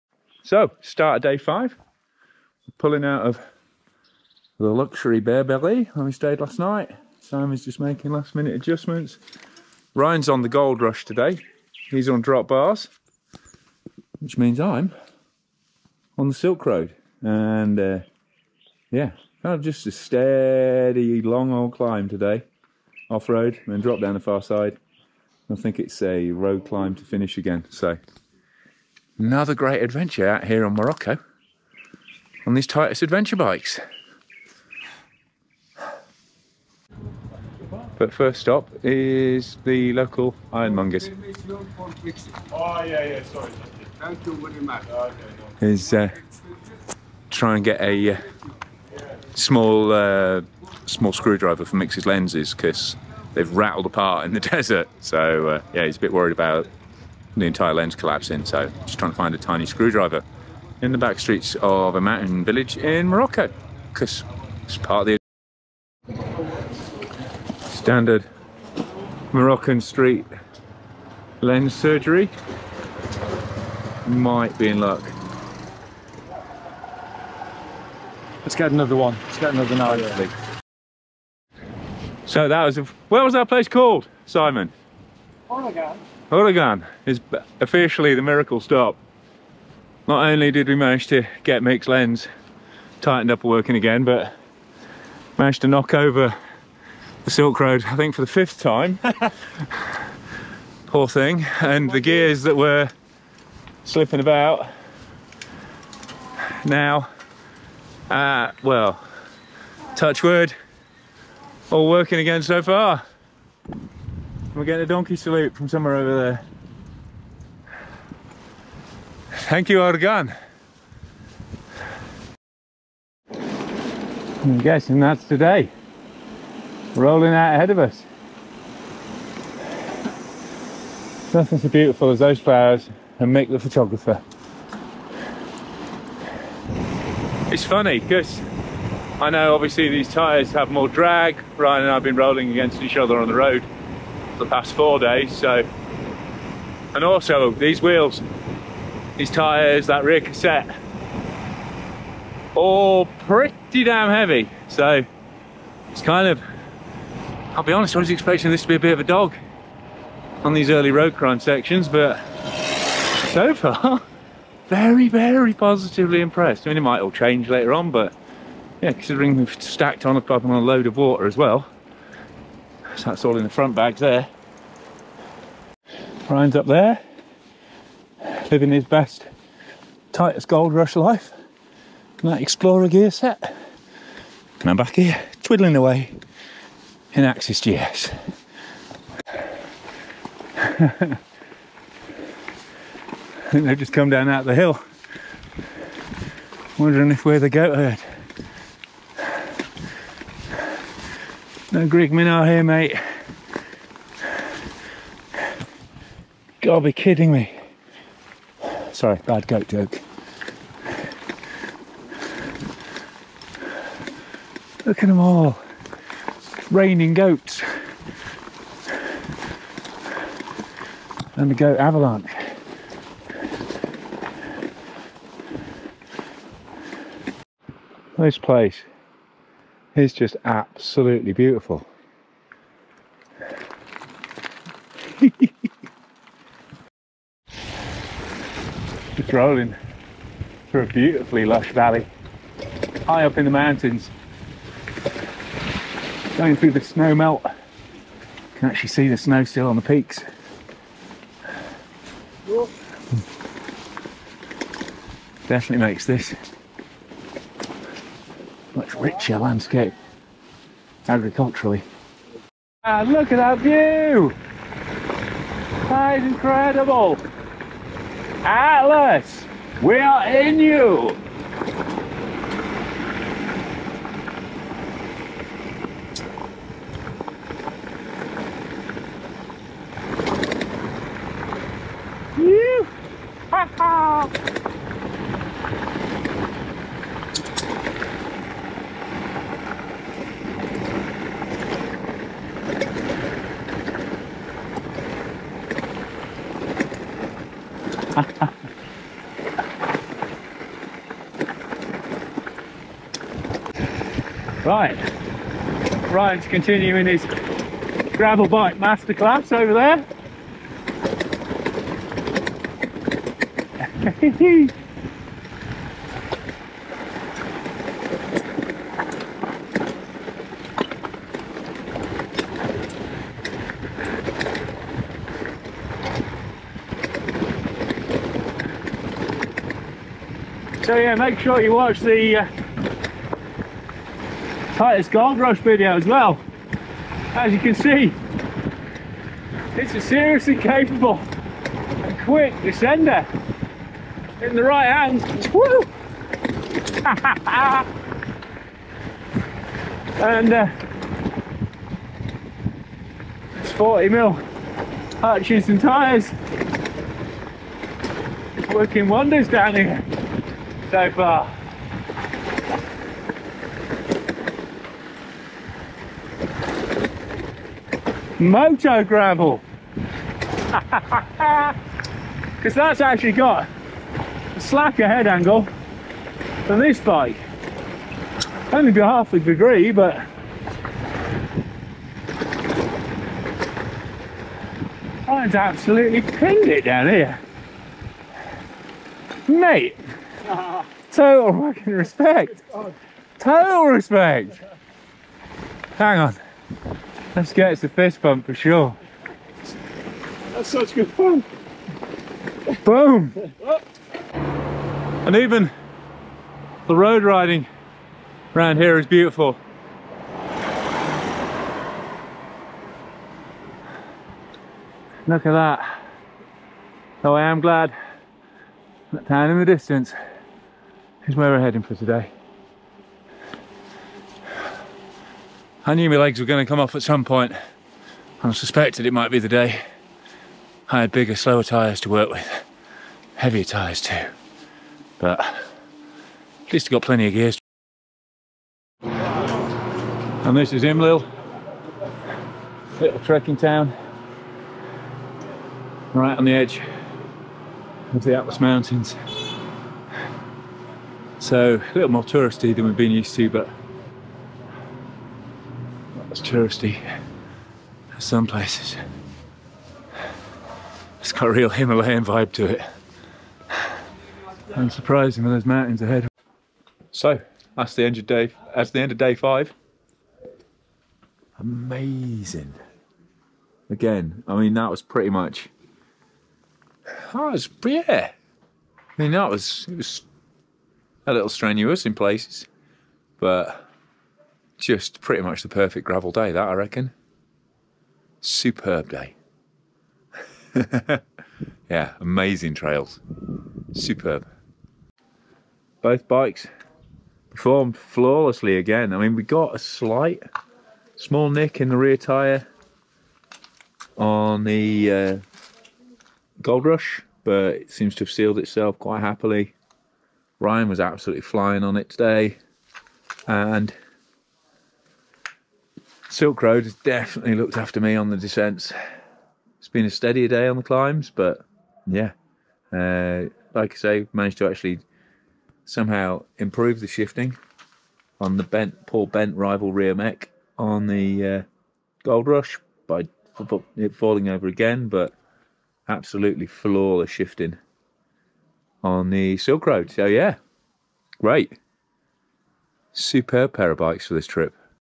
Titus Bikes African Adventure: PT 5 - Morrocan Chamonix
Join me and my mates exploring new gravel routes in Morocco on Titus titanium Goldrush and Silk Road adventure bikes with Global Bike Adventures. Part 5 was another big up and over day to Imlil and I properly died on the final climb. Lovely little trekking town underneath towering snow topped peaks that's known as 'Morocco's Chamonix' though. And I only threw up a bit in the bushes when we got there.
Thanks to Planet-X for sponsoring the filming and supplying us with the following bikes as well as Podsacs, Carnac and Zastrugi gear:

Any questions get busy in the comments below